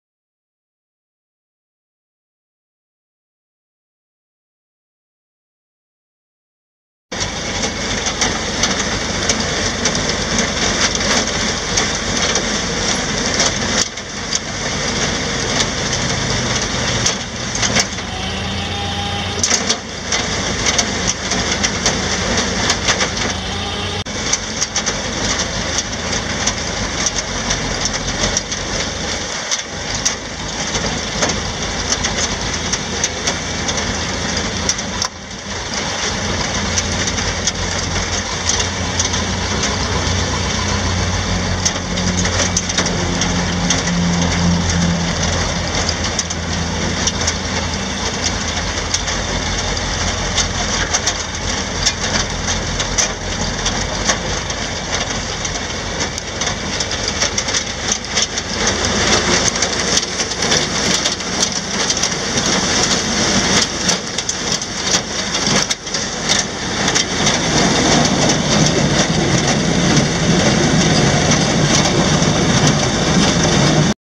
1979 Ford F600 feed truck for sale | sold at auction April 29, 2015
1979 Ford F600 feed truck
16,956 actual miles
370 C.I.D. V8 gas engine
Heat
AM/FM
Manual windows and locks
Vinyl interior
Lock-out hubs
Lever lock
John Deere 714A feeder wagon
20"W conveyor
Hydraulic driven
Serial W00714A012458
Front catwalk
Spring suspension
9.00-20 tires
GVWR 21,700 lbs
F66MVDJ1998

It was located in Riley, KS, and we sold it to the highest bidder for $9,020.